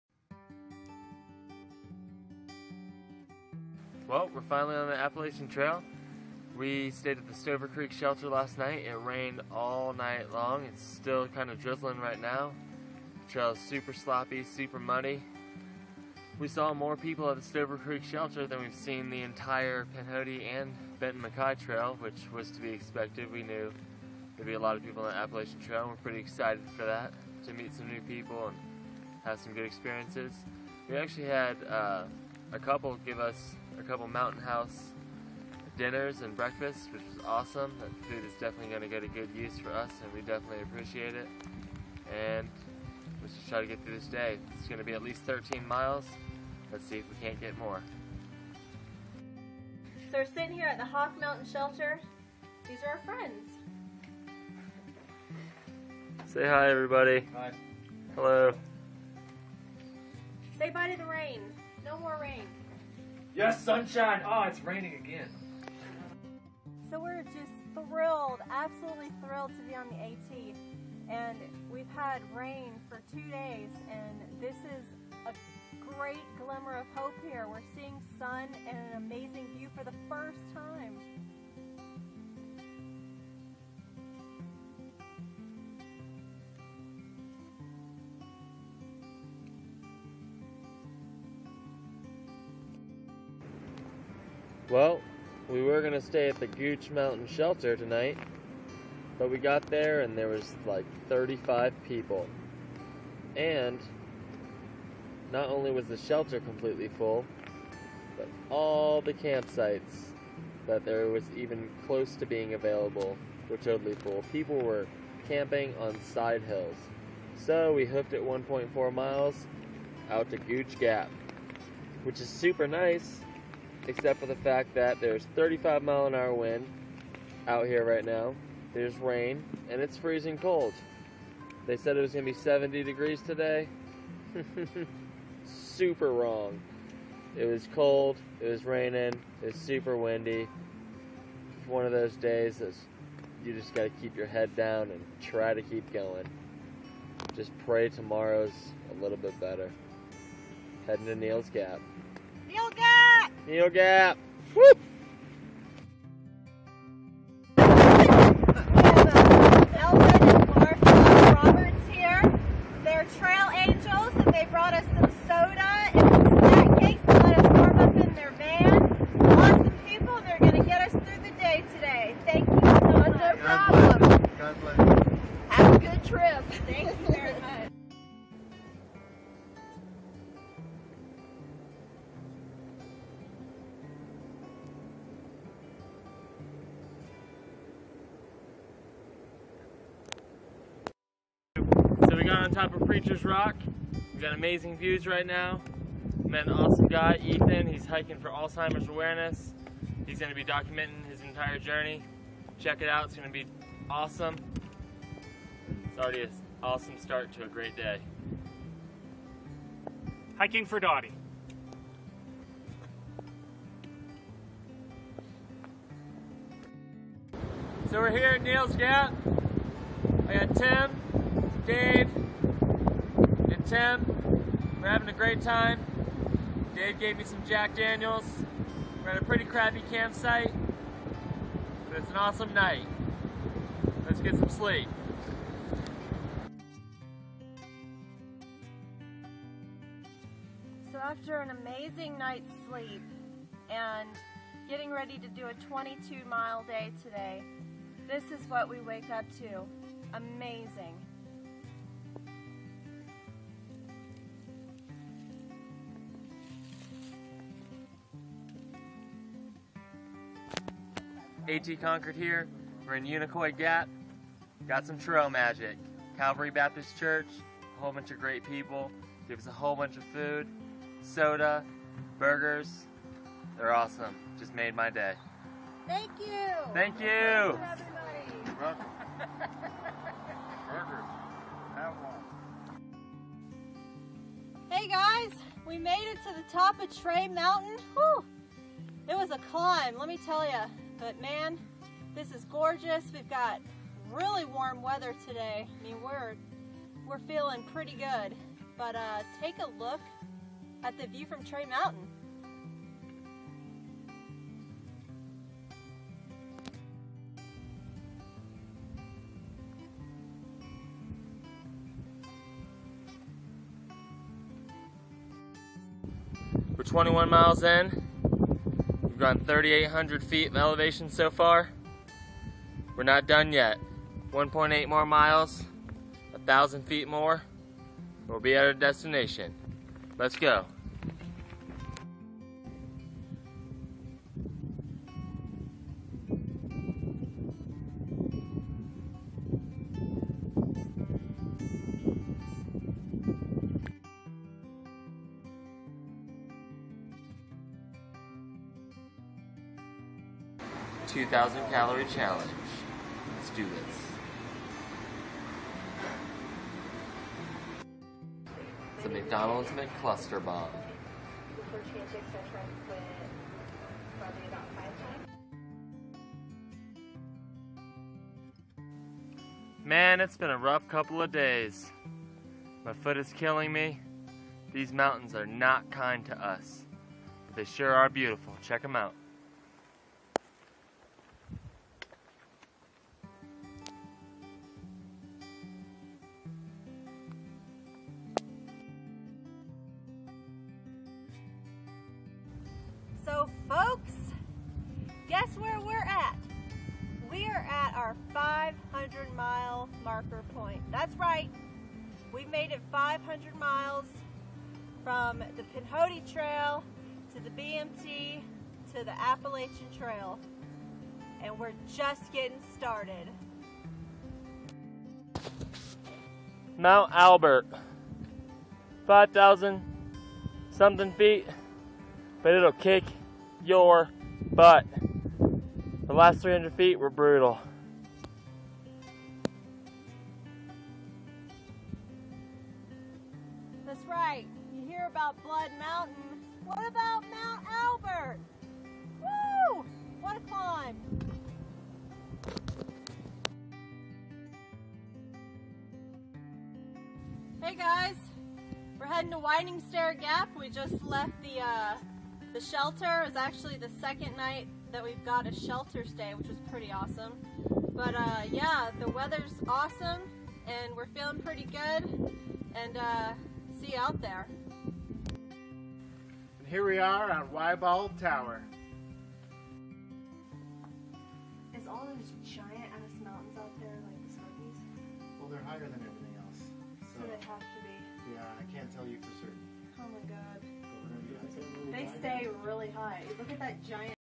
ATConquered Pinhoti and Appalachian Trail Update 8
Update 9 Day 29 - Day 35.  Starting the Appalahian Trail.